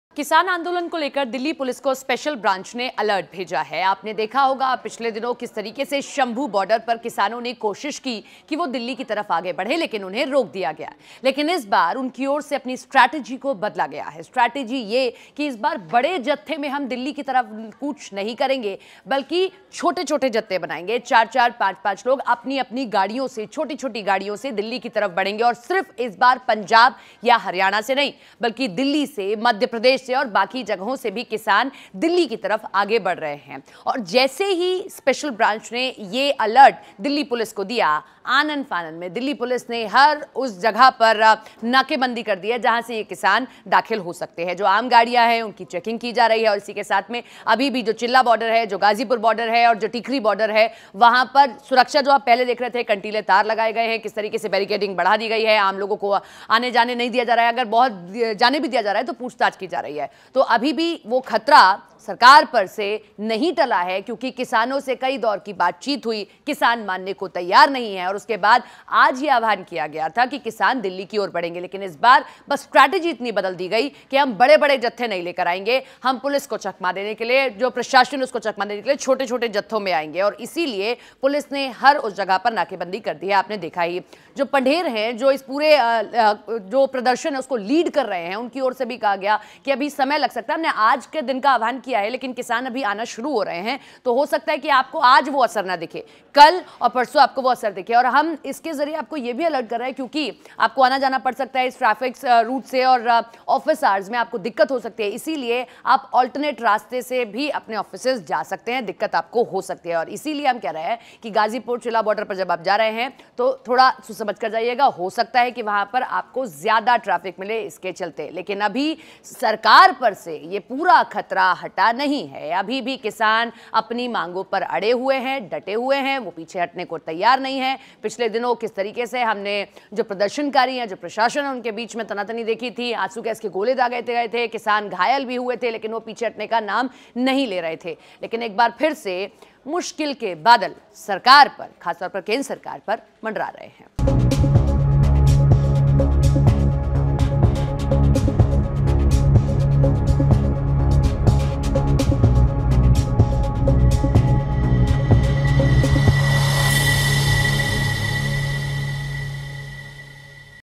Farmer Protest News : किसानों ने बैठक के बाद किया दिल्ली कूच का ऐलान ? | Hindi News | Delhi Morcha
बस-कार से आएंगे किसान. दिल्ली कूच पर अलर्ट, पुलिस ने आनन-फानन में उठाया यह कदम, किसान आंदोलन को लेकर दिल्ली पुलिस को स्पेशल ब्रांच ने अलर्ट भेजा है, जिसके बाद से आनन-फानन में सुरक्षा बढ़ा दी गई है. दरअसल, दिल्ली पुलिस को स्पेशल ब्रांच ने जो अलर्ट जारी किया है, उसके मुताबिक प्रदर्शनकारी किसान बस, कार और छोटी गाड़ियों से दिल्ली में घुस सकते हैं.